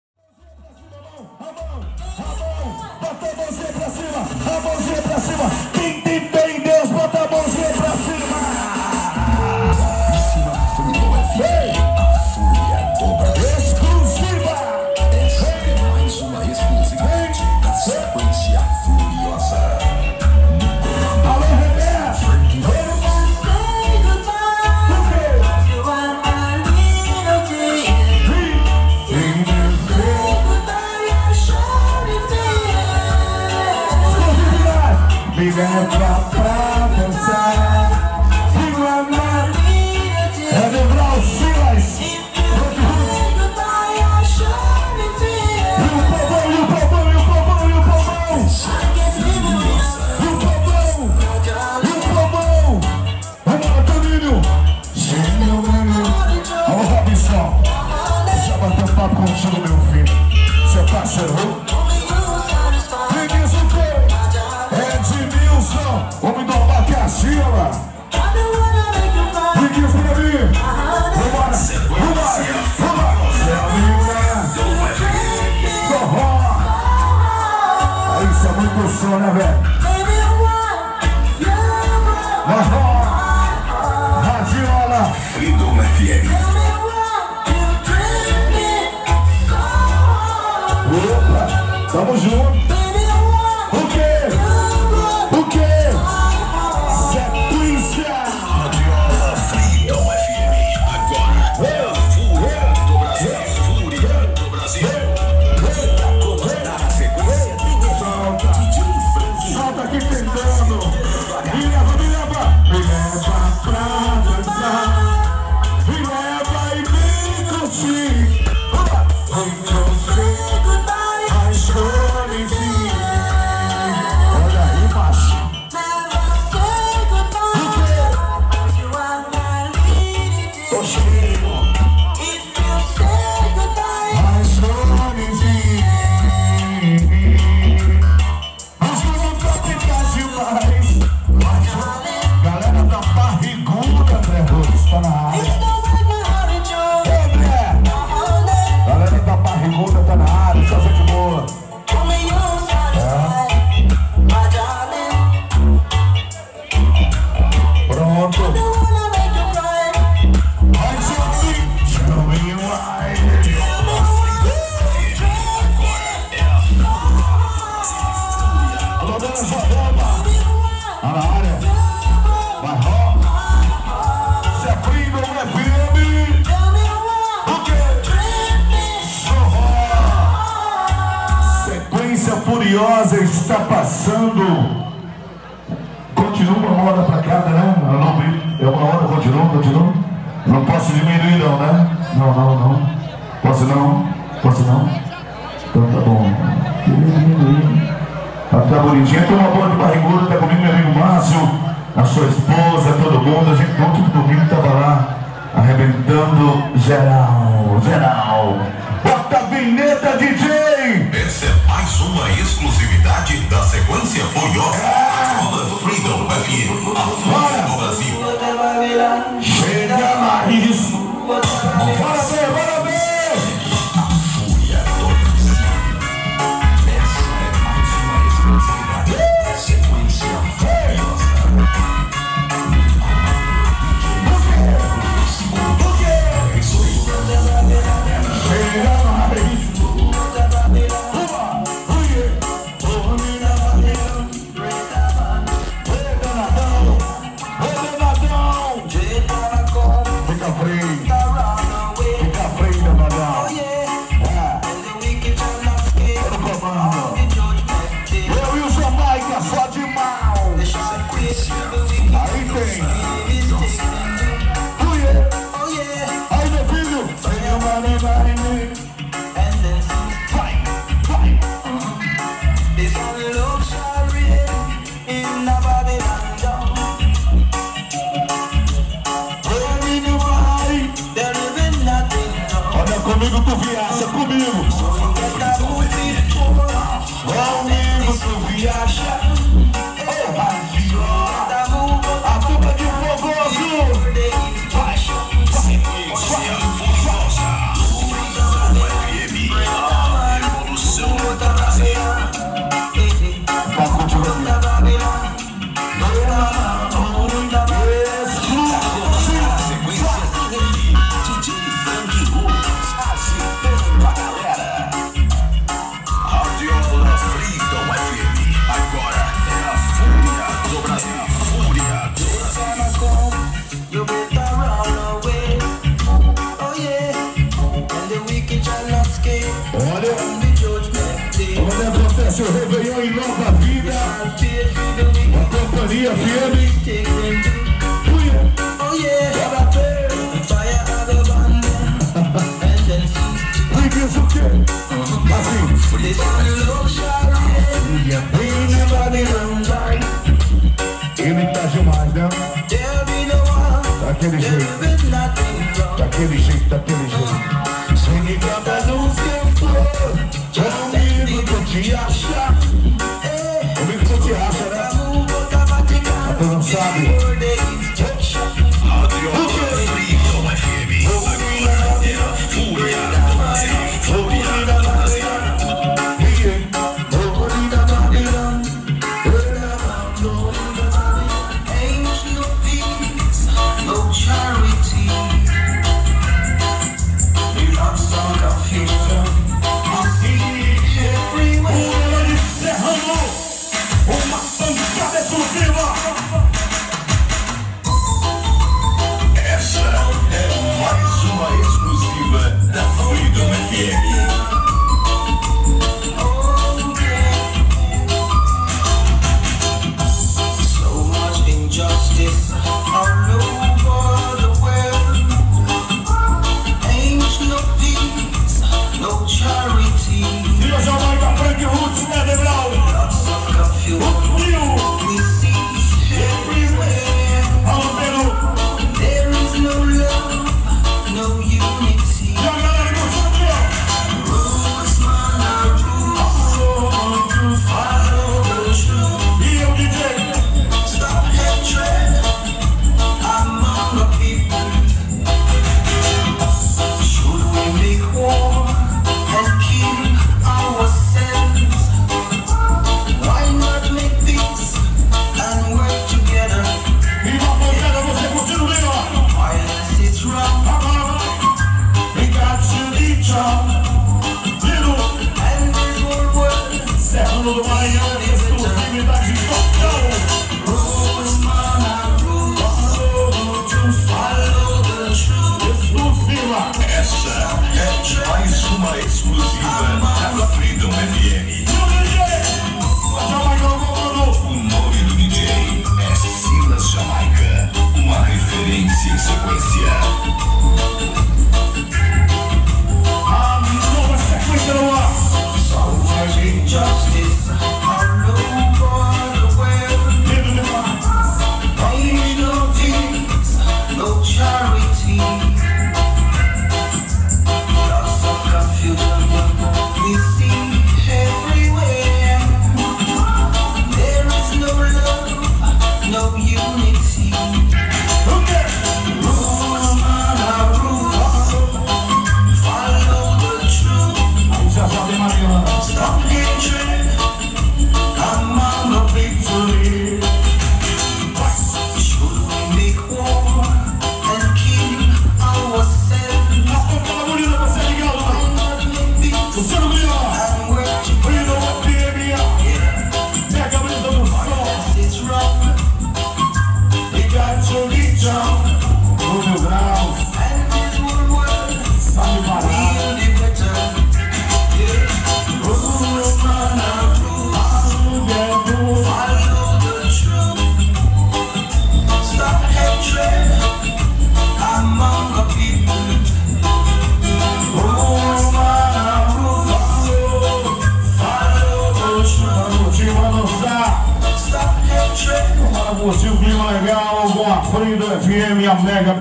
FESTA DO GUARANI EM MARENGO // radiola Freedom FM
ANIVERSÁRIO DO TIME GUARANI EM MARENGO SANTA RITA COM AS RADIOLAS MEGA BRISA DO SOM E FREEDOM FM